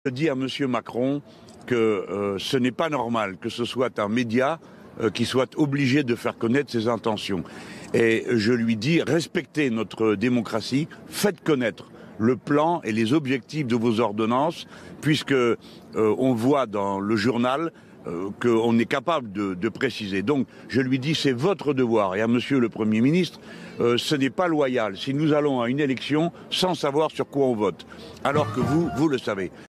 Jean-Luc Mélenchon somme Emmanuel Macron de s'expliquer sur ses ordonnances
En campagne à Toulouse, Jean-Luc Mélenchon s'agace de devoir apprendre dans la presse les intentions d'Emmanuel Macron sur la future réforme du Code du travail. À six jours des élections législatives, il somme le président et son gouvernement de s'expliquer sur les ordonnances qu'il souhaite faire passer.